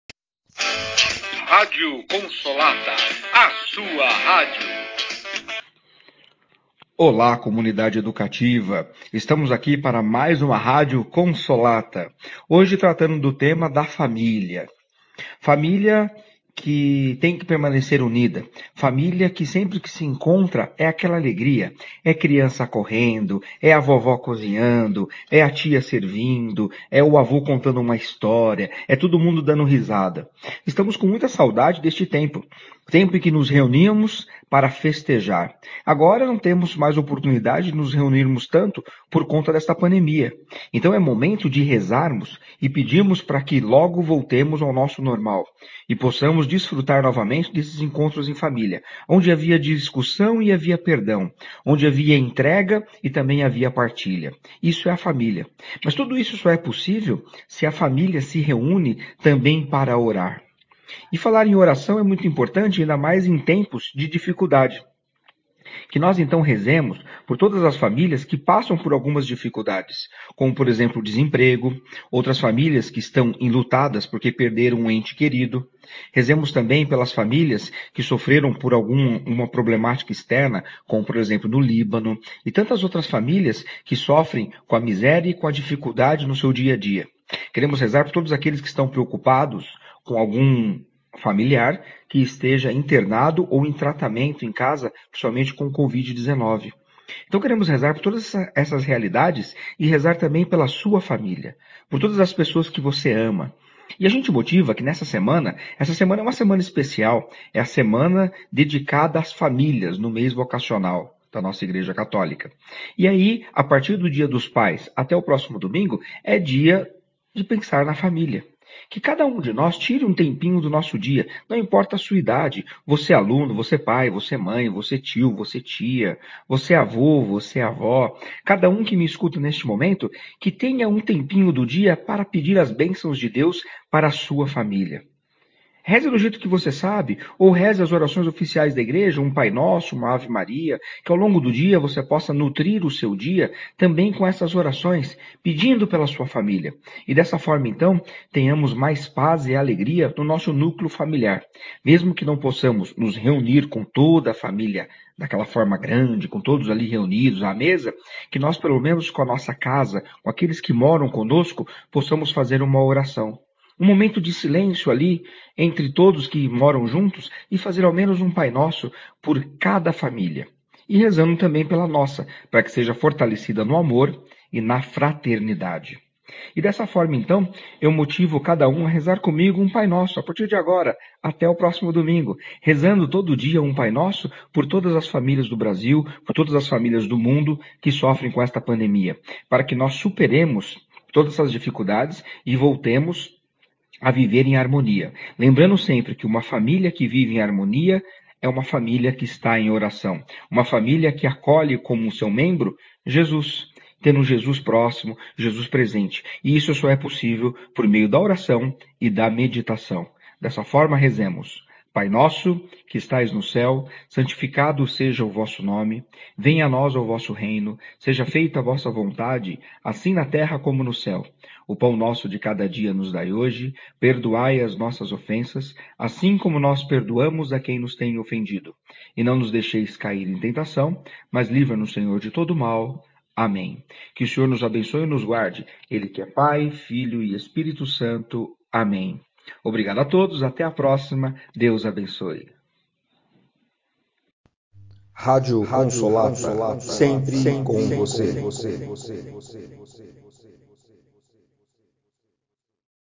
Rádio Consolata - 12/08
Querida comunidade educativa, paz e bem sempre!
Feliz Semana da Família!

Vamos ouvir a Rádio Consolata desta semana.
Continuamos a refletir sobre as temáticas do Mês Vocacional. Nesta semana, o professor Diego Scarmeloto nos fala sobre a família.
Seguimos unidos em prece para alcançarmos as graças que mais necessitamos, especialmente a vitória na luta contra o coronavírus e a descoberta da tão sonhada vacina.

"Uma comunidade que permanece unida realizará grandes obras."
Bem-aventurado Padre José Allamano